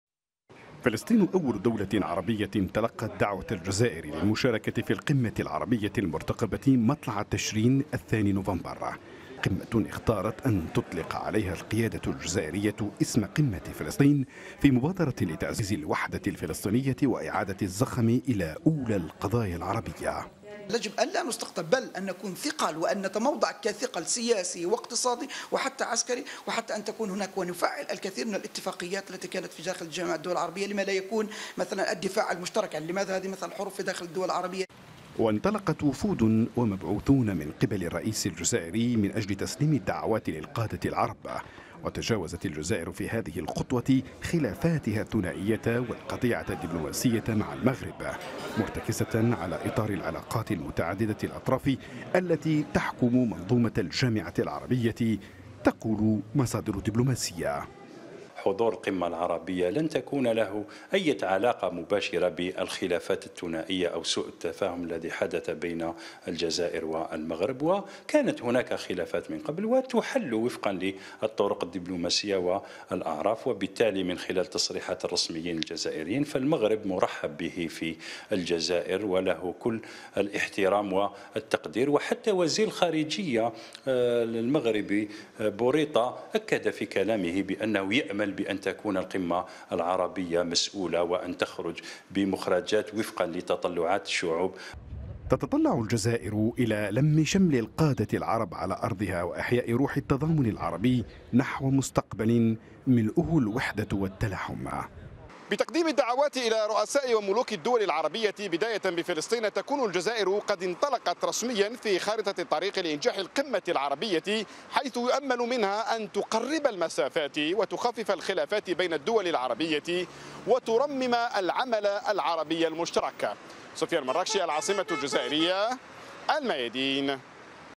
الجزائر تشرع بإرسال الدعوات للمشاركة في القمة العربية المرتقبة
شرعت الجزائر في إرسال الدعوات للمشاركة في القمة العربية المرتقبة مطلع تشرين الثاني نوفمبر المقبل، واختارت القيادة الجزائرية تقديم أول دعوة الى فلسطين ومصر على أن ترسل مبعوثاً للملك المغربي لدعوته للمشاركة، في خطوة ترمي إلى تجاوز الخلافات الثنائية واحترام ميثاق الجامعة العربية.

إعداد: سفيان مراكشي